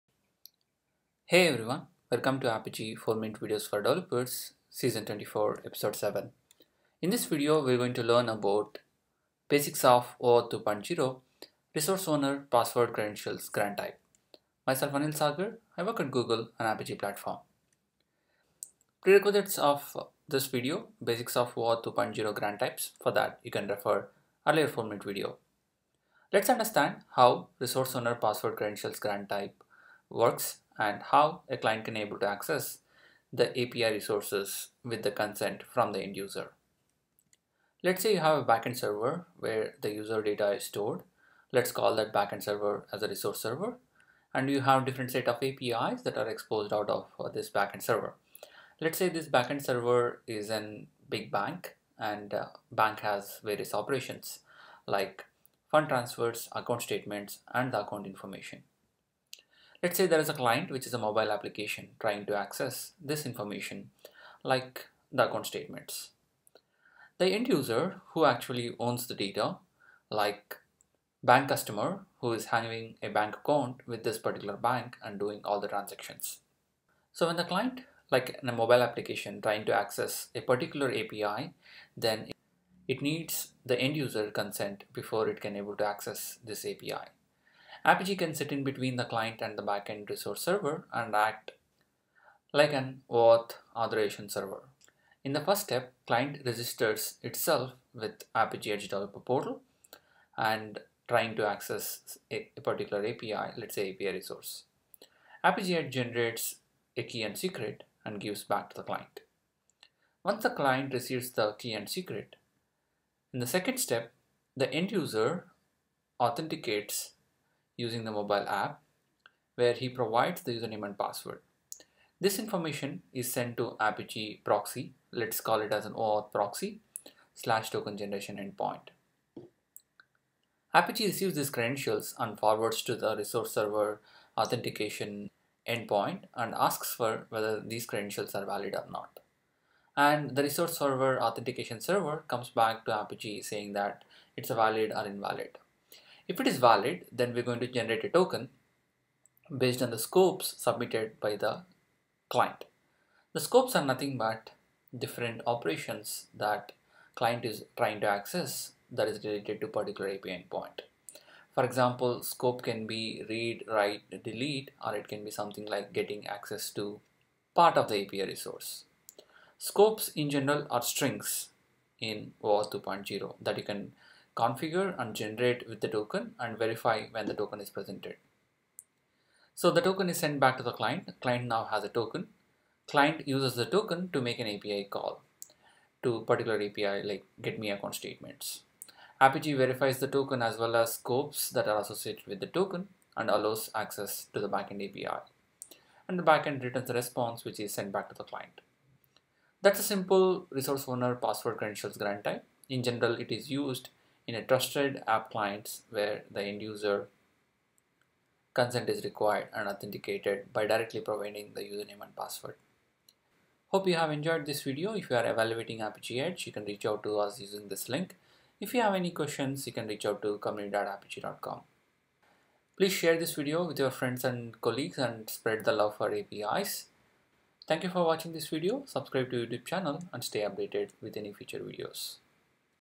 Apigee - 4MV4D - API Security - OAuth 2.0 - Resource Owner Password Credentials Overview - S24E07
In this Apigee Edge 4MV4D, Find out different steps involved in OAuth 2.0 Resource Owner Password Credentials GrantType & How Client gets access to APIs using API Tokens.